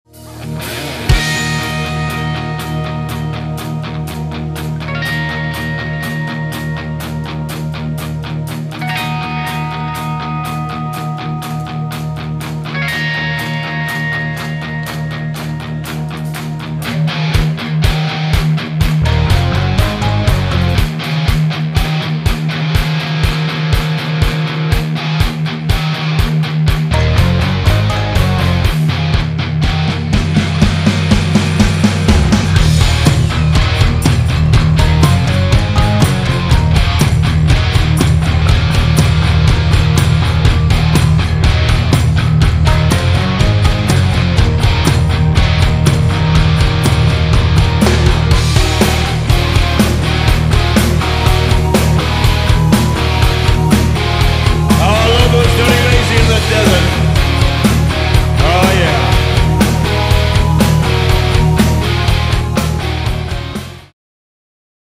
Graham Bonnet and Michael Men Band
Официальный трейлер к выходу концерта на DVD.